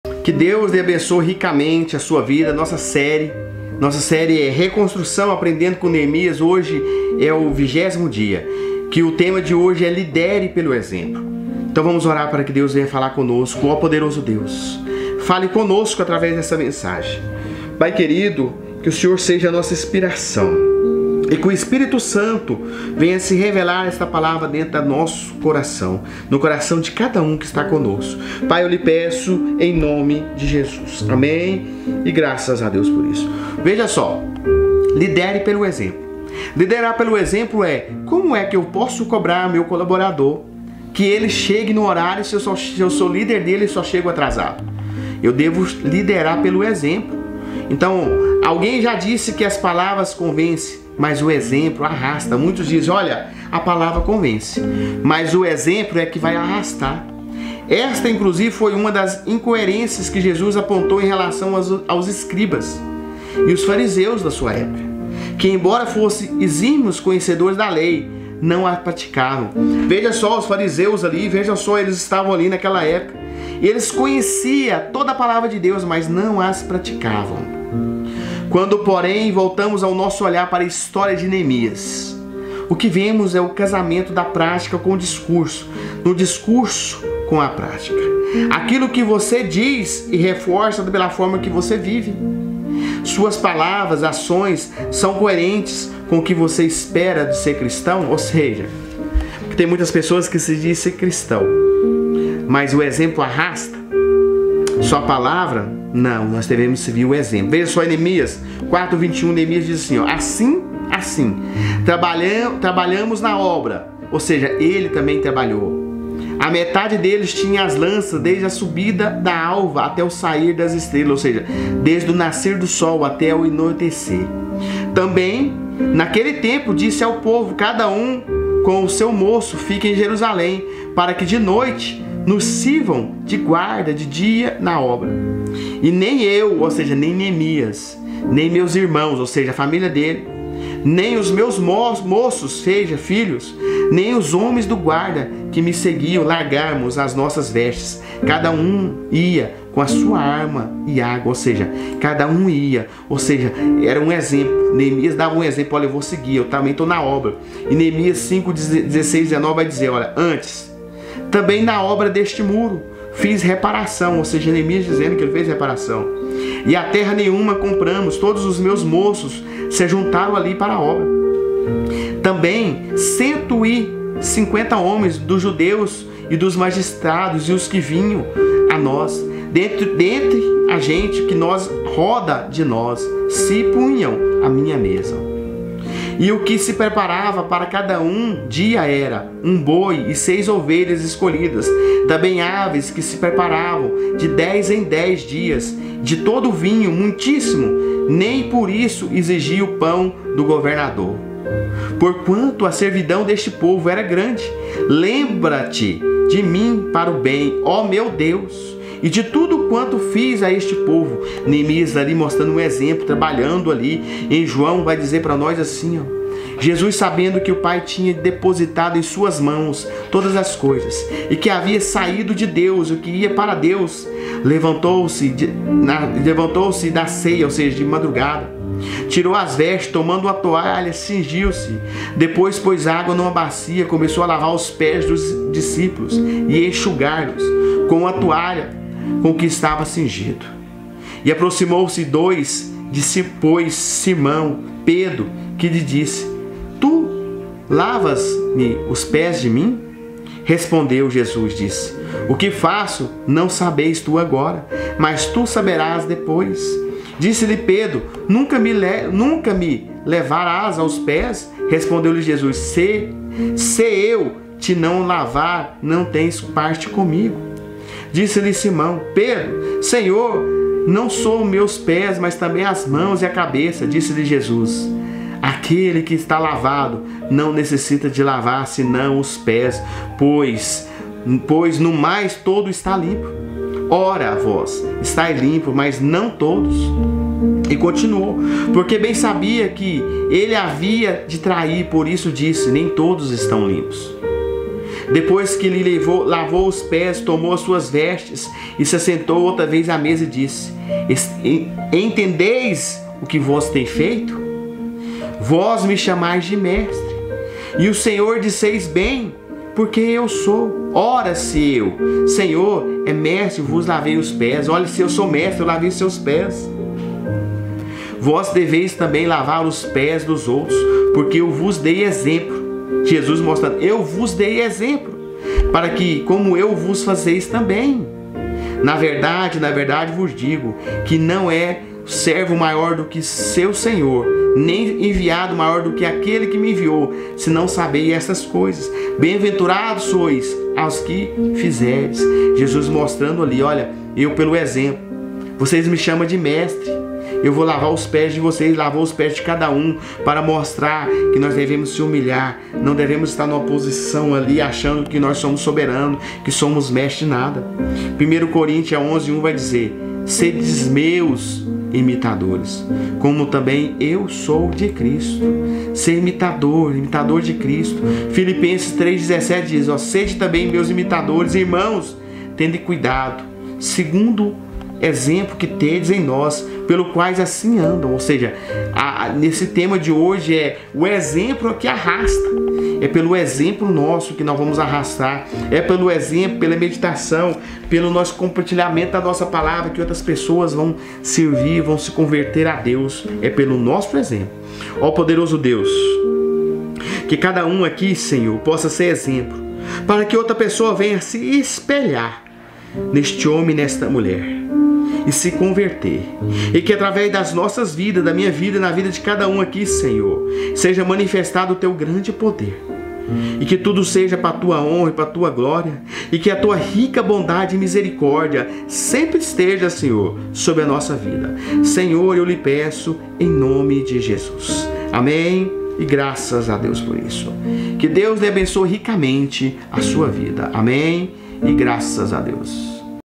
Série | Reconstrução: Aprendendo Com Neemias | Lidere Pelo Exemplo 20/22 | Pr. Rodrigo Morais
Série | Reconstrução: Aprendendo Com Neemias | Lidere pelo exemplo 20/22 com o Pr. Rodrigo Morais.

Devocional

Lidere Pelo Exemplo

Alguém já disse que as palavras convencem, mas só o exemplo arrasta. Essa inclusive foi uma das incoerências que Jesus apontou em relação aos escribas e fariseus de Sua época, que embora fossem exímios conhecedores da lei, não a praticavam.

Quando porém, voltamos nosso olhar para a história de Neemias, o que vemos é o casamento da prática com o discurso e do discurso com a prática.

Aquilo que você diz crer é reforçado pela forma como você vive? Suas palavras e ações são coerentes com o que se espera de um cristão?

Assim trabalhávamos na obra; e metade deles tinha as lanças desde a subida da alva até ao sair das estrelas.
Também naquele tempo disse ao povo: Cada um com o seu servo fique em Jerusalém, para que à noite nos sirvam de guarda, e de dia na obra.
E nem eu, nem meus irmãos, nem meus servos, nem os homens da guarda que me seguiam largávamos as nossas vestes; cada um tinha suas armas e água.
Neemias 4:21-23

Como também na obra deste muro fiz reparação, e terra nenhuma compramos; e todos os meus servos se ajuntaram ali à obra.
Também dos judeus e dos magistrados, cento e cinqüenta homens, e os que vinham a nós dentre as nações que estão ao redor de nós, se punham à minha mesa.
E o que se preparava para cada dia era um boi e seis ovelhas escolhidas; também aves se me preparavam e, de dez em dez dias, muito vinho de todas as espécies; e nem por isso exigi o pão do governador, porquanto a servidão deste povo era grande.
Lembra-te de mim para bem, ó meu Deus, e de tudo quanto fiz a este povo.
Neemias 5:16-19

Jesus, sabendo que o Pai tinha depositado nas suas mãos todas as coisas, e que havia saído de Deus e ia para Deus,
Levantou-se da ceia, tirou as vestes, e, tomando uma toalha, cingiu-se.
Depois deitou água numa bacia, e começou a lavar os pés aos discípulos, e a enxugar-lhos com a toalha com que estava cingido.
Aproximou-se, pois, de Simão Pedro, que lhe disse: Senhor, tu lavas-me os pés a mim?
Respondeu Jesus, e disse-lhe: O que eu faço não o sabes tu agora, mas tu o saberás depois.
Disse-lhe Pedro: Nunca me lavarás os pés. Respondeu-lhe Jesus: Se eu te não lavar, não tens parte comigo.
Disse-lhe Simão Pedro: Senhor, não só os meus pés, mas também as mãos e a cabeça.
Disse-lhe Jesus: Aquele que está lavado não necessita de lavar senão os pés, pois no mais todo está limpo. Ora vós estais limpos, mas não todos.
Porque bem sabia ele quem o havia de trair; por isso disse: Nem todos estais limpos.
Depois que lhes lavou os pés, e tomou as suas vestes, e se assentou outra vez à mesa, disse-lhes: Entendeis o que vos tenho feito?
Vós me chamais Mestre e Senhor, e dizeis bem, porque eu o sou.
Ora, se eu, Senhor e Mestre, vos lavei os pés, vós deveis também lavar os pés uns aos outros.
Porque eu vos dei o exemplo, para que, como eu vos fiz, façais vós também.
Na verdade, na verdade vos digo que não é o servo maior do que o seu senhor, nem o enviado maior do que aquele que o enviou.
Se sabeis estas coisas, bem-aventurados sois se as fizerdes.
João 13:3-17

Sede meus imitadores, como também eu de Cristo.
1 Coríntios 11:1

Sede também meus imitadores, irmãos, e tende cuidado
Filipenses 3:17

Oração: Deus, a começar em mim, que eu seja moldado segundo a Sua

AG: 1626
CC: 5025-8
OP: 001